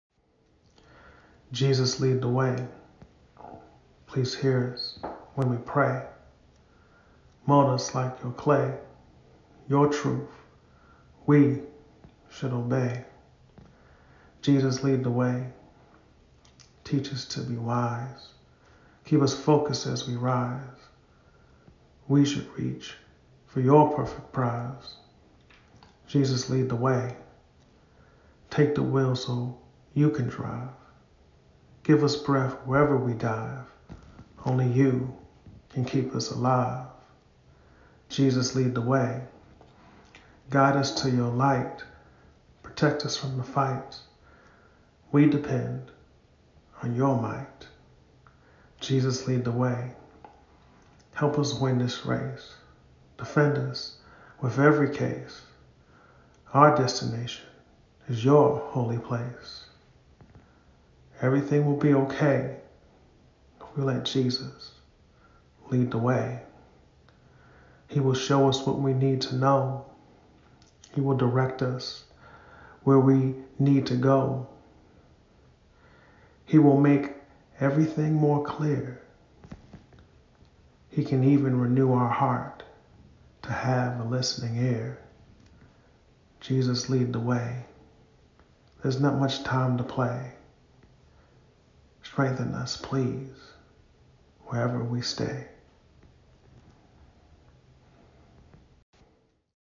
Jesus Lead the Way by poet Andrew Richardson Jr
Jesus Lead the Way

Please hear us when we pray.
Mold us like your clay.
Your truth we should obey.

Teach us to be wise.
Keep us focused as we rise.
We should reach for your perfect prize.

Take the wheel so you can drive.
Give us breath wherever we dive.
Only you can keep us alive.

Guide us to your light.
Protect us from the fights.
We depend on your might.

Help us win this race.
Defend us with every case.
Our destination is your holy place.

Everything will be okay if you let Jesus lead the way.
He will show what you need to know.
He will direct where you need to go.
He will make everything more clear.
He can even renew your heart to have a listening ear.

There's not much time to play.
Strengthen us please wherever we stay.